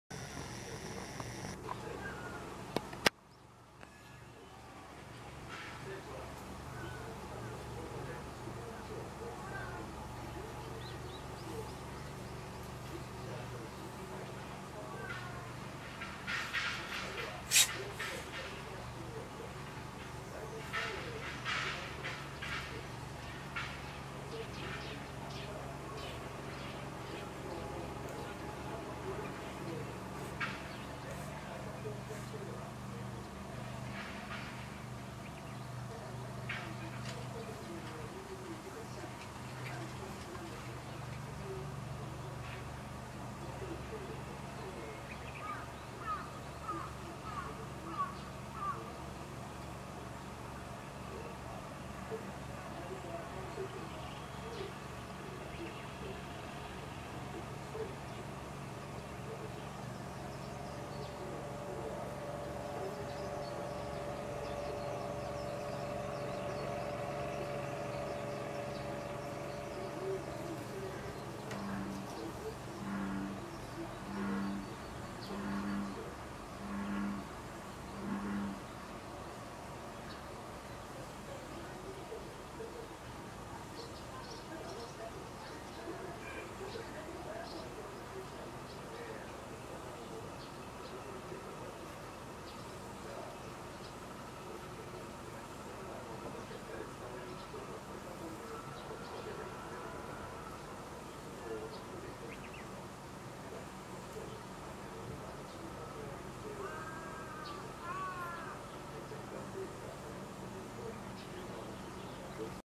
仔猫が大猫に戦いを挑むが
仔猫が猫相の悪い大猫に挑みますが・・母猫にじゃれているだけです。軽くいなされ、母猫も尻尾を振って遊ばせます。近所から引っ越してきた野良猫親子です。